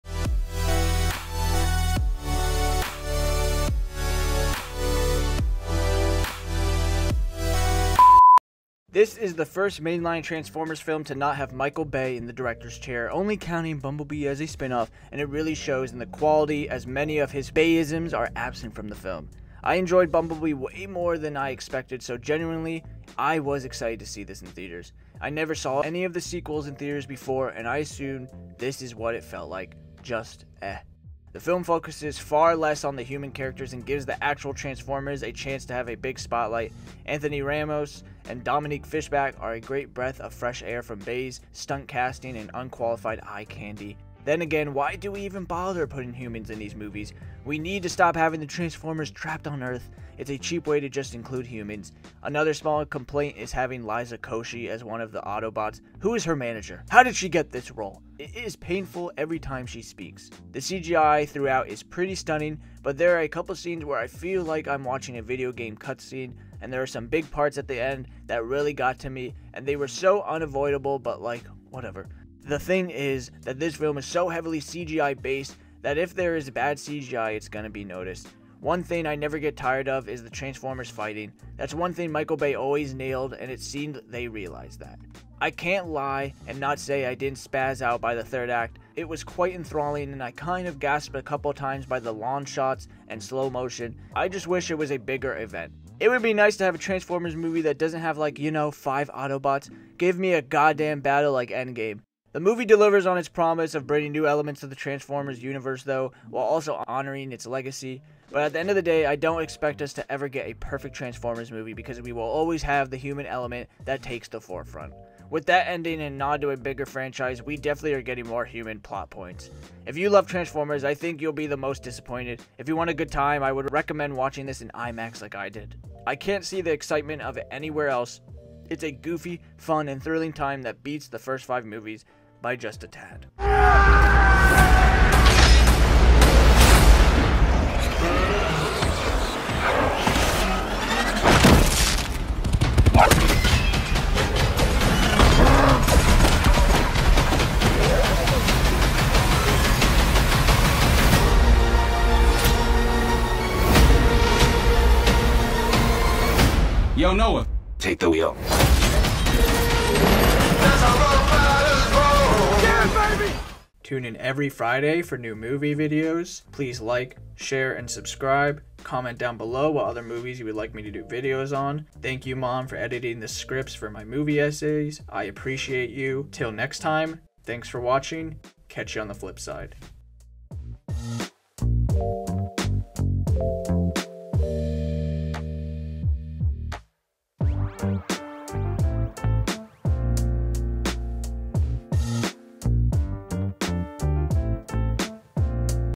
transformers rise of the beasts: mindless action (movie review)
AEAEAEAEAE
Letterboxd: @mrhonthy
Written & Edited by
Chris Honthy
Co-Written by
Christina Honthy

If you don't like the movie, then just don't watch my video :)